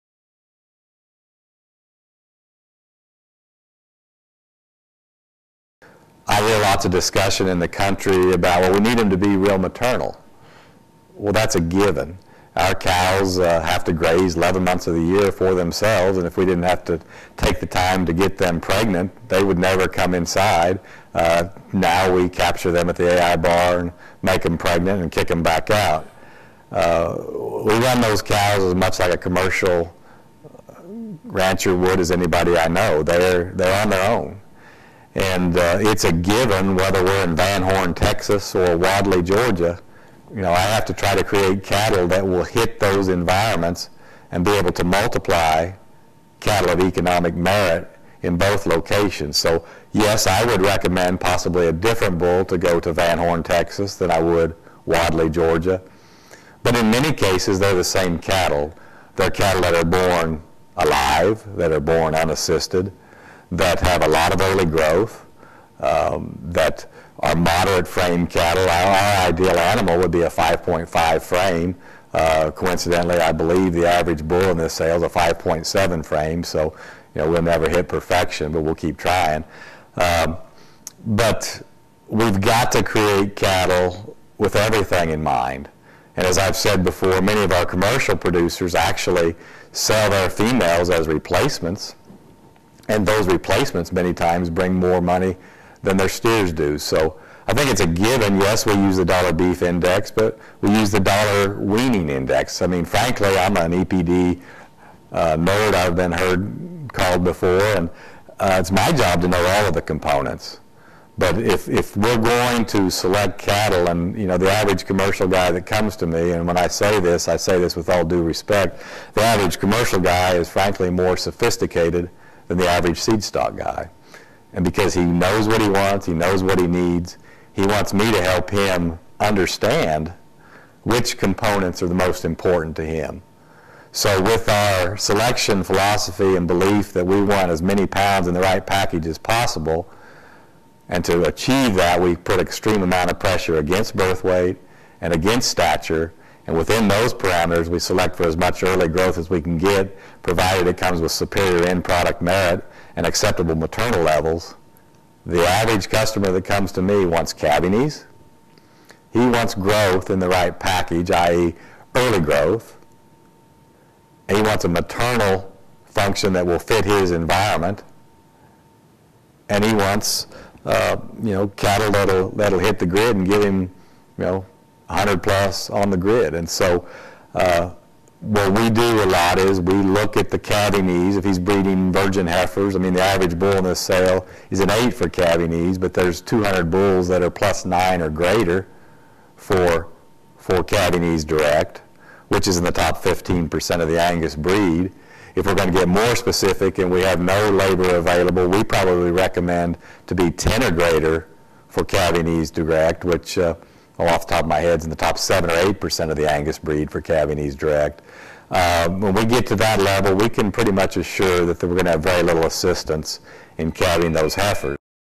Designing Cattle For Your Environment
Mark explains the considerations for production criteria at Gardiner Angus Ranch and meeting the various needs of customers across the country.